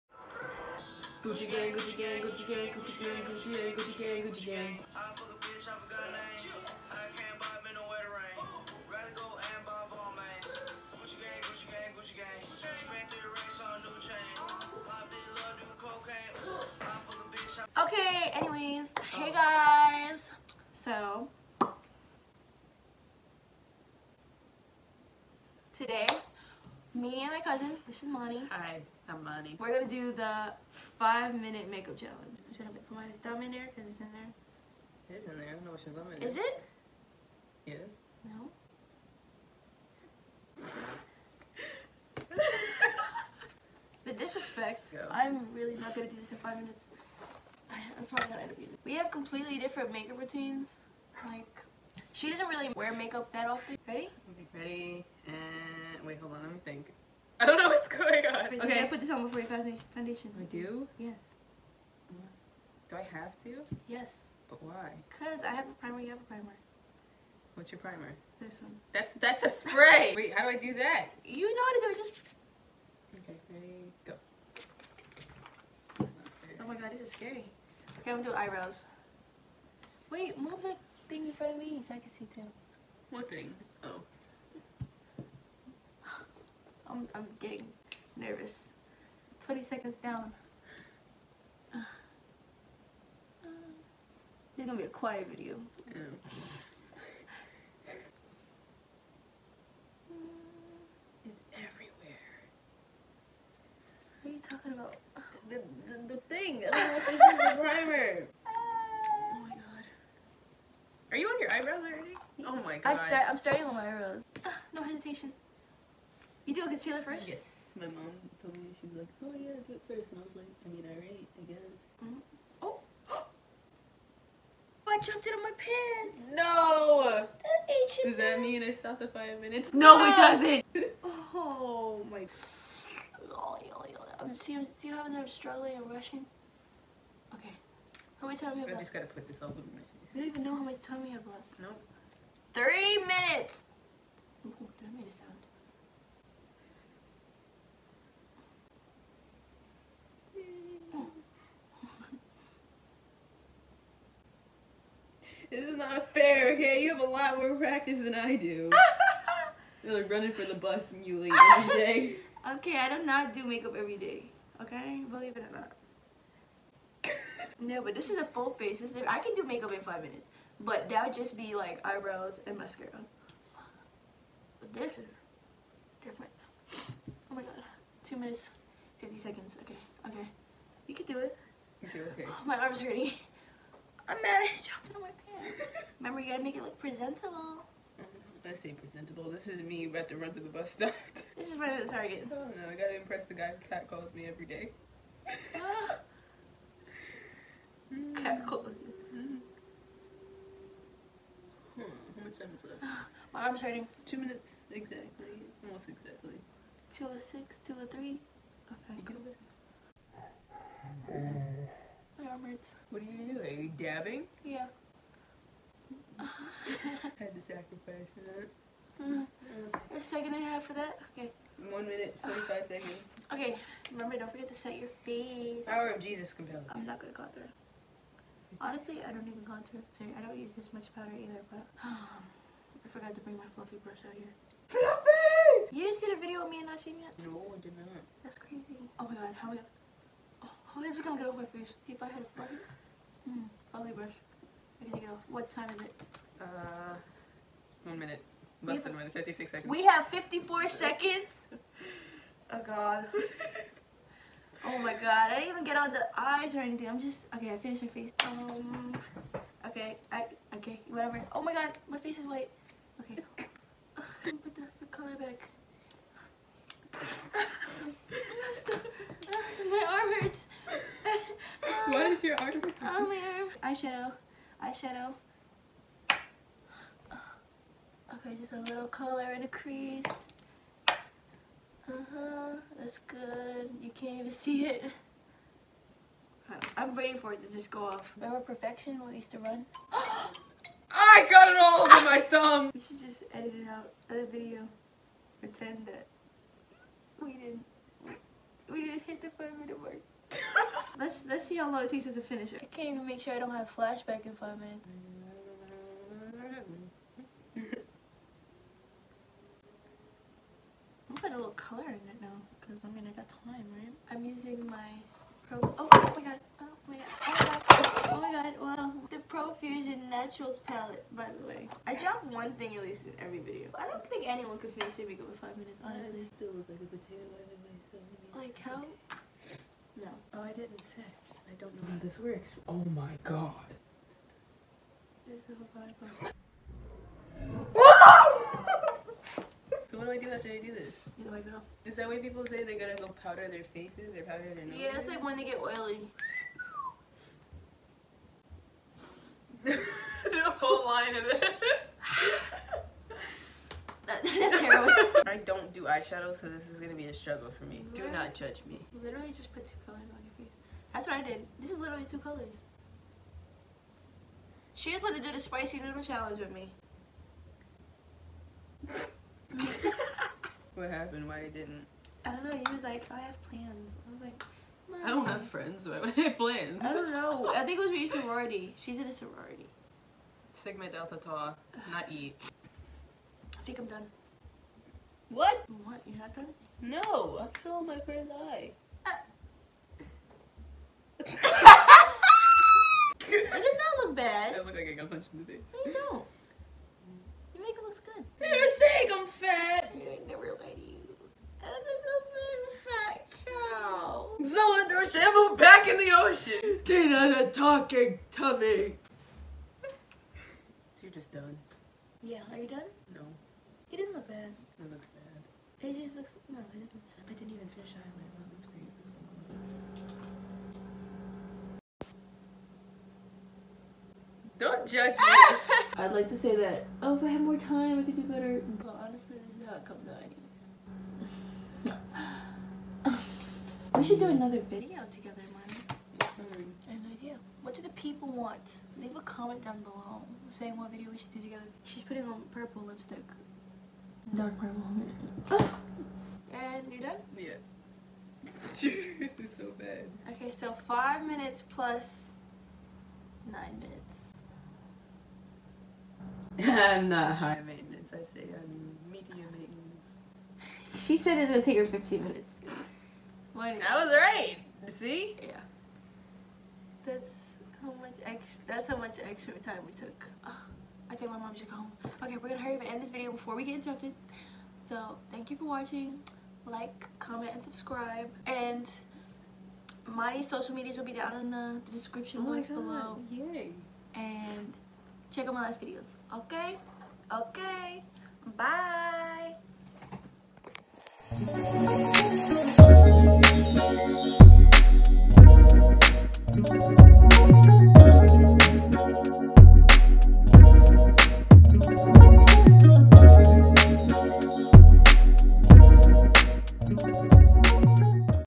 FIVE MINUTE MAKEUP CHALLENGE WITH MY COUSIN // SUCH A FAIL LMAO // TIANI MCLOYD
...Just watch.

IMANI'S SOCIALS

Snap// - @maniii_28

SOCIAL MEDIAS

Snap// @w5nderland